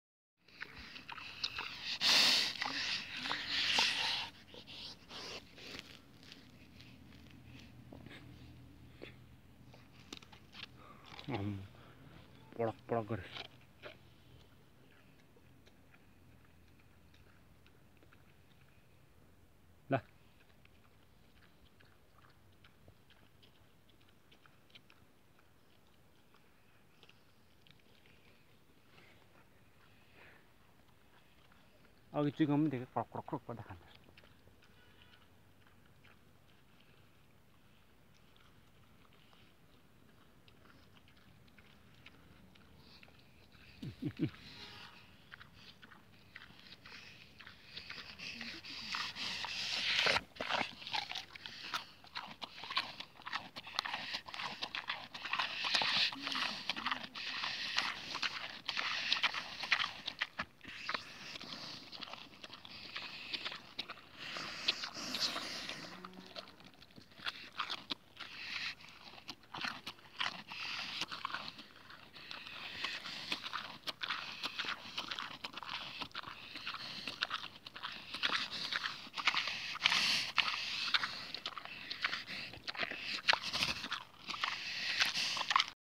Himalayan Jungle Boar
Most ever closest video of Boar by tshiteez gurung